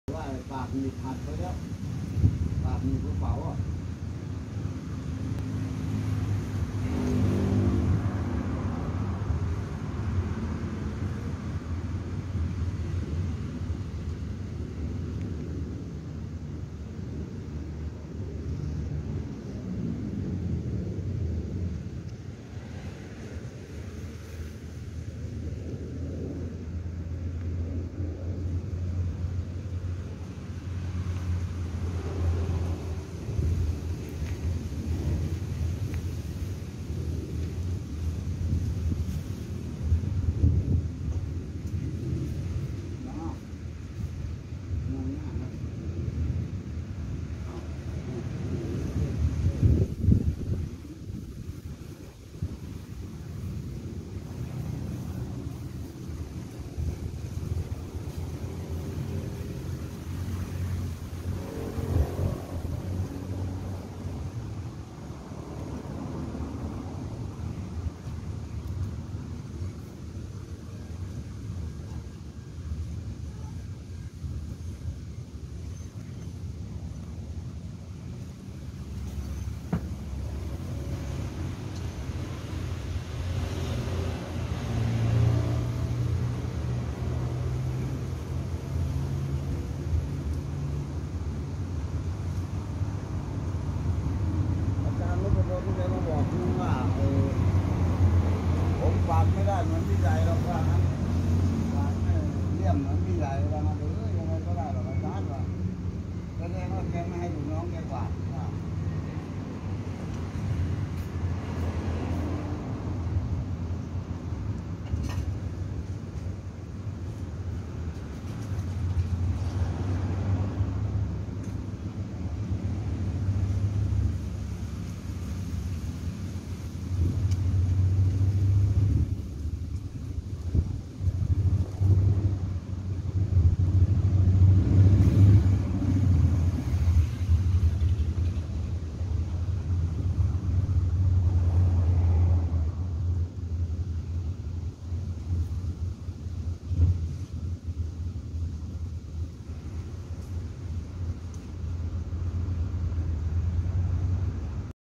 Ep.35ดูหอยทาก🐌ชีวิตหอยทาก🐌snail
มาดูหอยทากตัวนี้ห้อยโหนตัว อยู่บนต้นไม้  น่าจะหาที่นอน เพราหอยทากนอกจากจะมุดดินแล้ว บางสายพันธุ์ ชอบปีนต้นไม้นอน